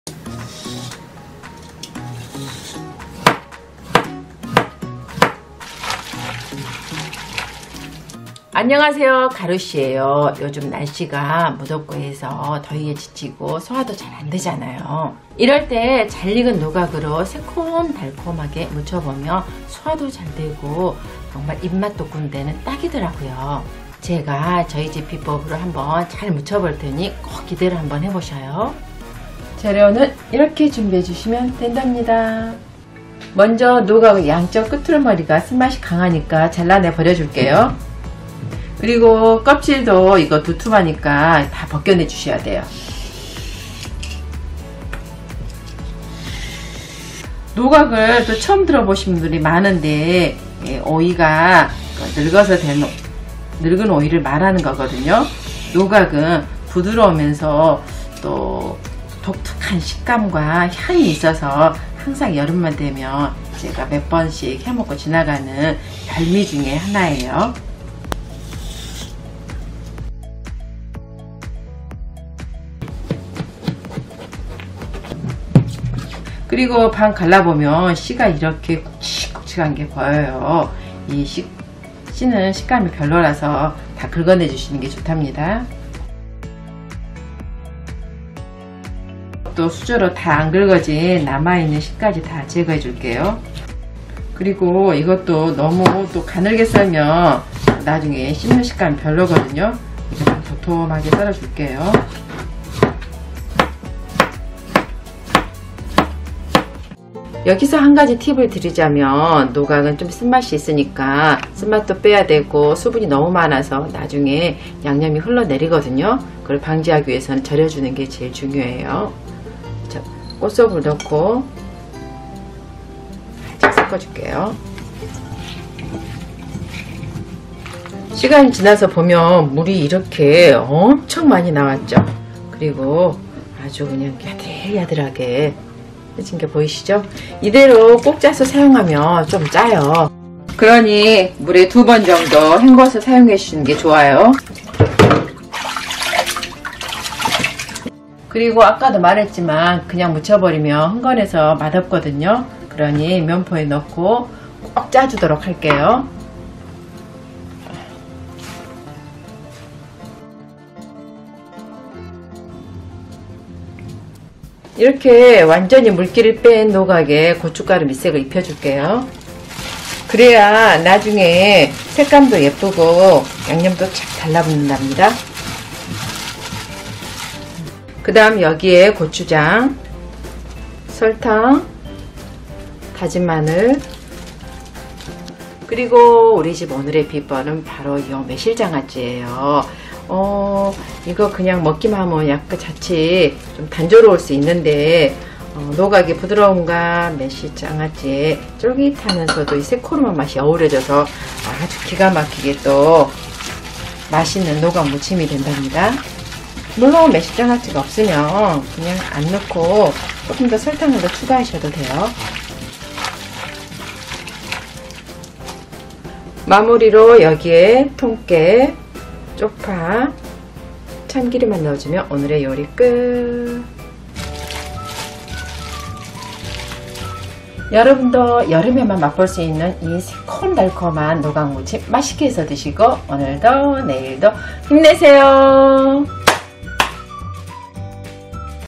노각무침에 딱! 이것만 넣으면 왜이렇게 맛있어? 역시 여름반찬으론 늙은오이가 최고죠!
재료: 노각 1.3kg,
꽃소금 2T(30g), 쪽파 한두 줄기
양념: 고추장 3T, 고춧가루 1T,
매실장아찌 3T(55g), 다진 마늘 1T,
설탕 1t, 통깨 1/2T, 참기름 1t

조리시간: 25분  난이도: ★★☆☆☆
*3인 가족 2끼 분량입니다.
T = 15cc, t = 5cc 계량 수저 기준.

가족들 한줄평
엄마:첫 촉감은 양념 무친 회 느낌이 드는데.
오독오독한 식감에서 오이 맛이 확! 난달까?
아빠: 양념도 적절하고, 식감도 잘 나와서 맛남.
딸: 오이와는 다른 익은 향이 독특한 느낌.
매콤달콤시원한게 밥 먹을 때 집어먹기 좋음.
총평:너무 맛나 또 찾게 될걸 약속드려요ㅎㅎ

[노각 손질 및 절이기]
1. 먼저 노각의 양 끝은 쓰니깐,
(특히 꼭지 부분 쪽) 도려내 버려주고
껍질도 질기고 쓴맛이 나기 때문에,
필러로 완벽하게 다 벗겨주는 게 좋아요.

2. 그다음 반 갈라보면 씨가
두껍게 있는 게 보이시죠.
이건 너무 자라서 쓴맛도 나고 맛없으니깐,
수저나 칼로 깨끗하게 도려내 버려주세요.
(남은 씨나 잔여물까지 쓱쓱 긁어 버려주면 OK.)

3. 또 나중에 절일 거니깐, 얇게 썰지 말고
0.5~0.7cm 정도로 조금 도톰하게 썰어야
나중에 오독오독하니 맛있답니다.

4. 그리고 노각이 오이 계열이다 보니깐
이게 그냥 무치기에는 수분이 너무 많으니.
꽃소금 2T를 넣고 잘 섞어준 다음,
15분 동안 절여주면 된답니다.
(그러면 쓴맛도 함께 빠져나오겠죠!)

5. 15분 후, 묻어있는 짠 기를 빼기 위해
넉넉한 물에 2번 정도 씻어준 다음
체에 밭쳐 물기를 빼주면 되는데.

6. 그래도 물기가 많으니깐,
면포에 넣고 최대한 꽉! 짜주세요.
(거의 탈수시킨다는 느낌으로?
이게 꽤 어깨가 많이 아프니깐
탈수기 있으시면 사용하셔도 될 듯.)

7. 이렇게 꽉 짜면 물기가 없어져서
무게가 500g으로 팍 줄어들더라고요.
(그래서 큰 노각 하나를 담가도,
그렇게 양이 많지 않답니다ㅎㅎ)

[늙은오이 노각무침 만드는법]
1. 그리고 이렇게 물기를 뺀 노각에
고춧가루를 넣고 조물조물 섞어서
밑간을 해주시면 된답니다.
*그래야 색감도 예쁘게 잘 나오고,
나중에 양념도 촥 달라붙거든요ㅎㅎ

2. 고추장, 매실장아찌, 설탕, 다진 마늘을 넣고
조물조물 잘 섞어주면 된답니다.
*매실장아찌가 오늘의 포인트!
은은한 단맛과 중간에 다른 식감을 더해줘
노각을 더욱 맛있게 먹게 도와준달까?!
물론 없으시면 매실청을 추가로 넣거나,
설탕양을 더 늘리셔도 괜찮아요ㅎㅎ

3. 마무리로 고명 재료인 실파와
통깨, 참기름을 넣고 한 번만 가볍게
섞어주면 오늘의 요리 끝.

보기만 해도 시원매콤하니
이건 정말 맛있어 보이죠♥
한번 절인 덕분에 오독오독한 식감과
노각 특유의 향만으로도 참 좋은데~
여기에 매콤달콤한 양념이 잘 어우러져
진짜 더위를 싹! 식혀주는 느낌이 들어
자꾸자꾸 손이 가는 매력 반찬이랍니다.

여러분도 시원한 노각무침 맛있게 해 드시고,
더운 여름에 모두 모두 힘내세요!!!